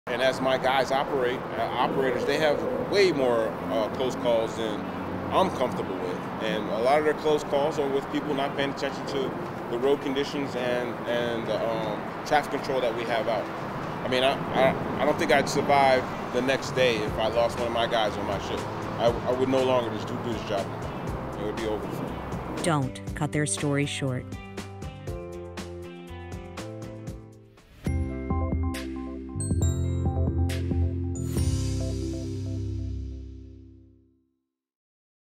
2: GDOT Work Zone Safety Week Trailer - HERO
GDOT celebrates 2021 Work Zone Awareness Week.  Check out our video trailer highlighting the work of HERO Unit Operators.